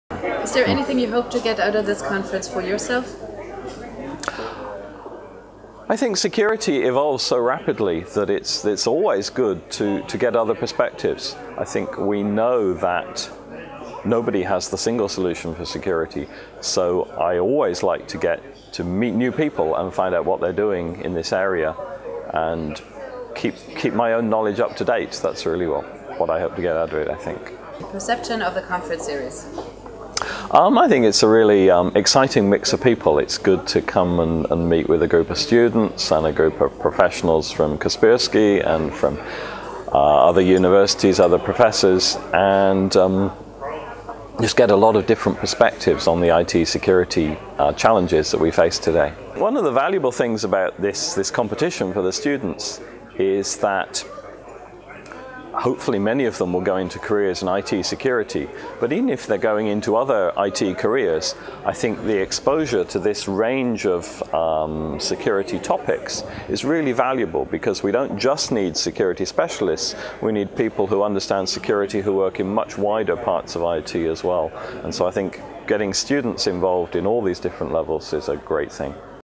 Dr. Andrew Martin's interview, "IT Security for the Next Generation" 2012
Dr. Andrew Martin, University Lecturer in Software Engineering, Department of Computer Science, University of Oxford; a Member of the Conference Program Committee.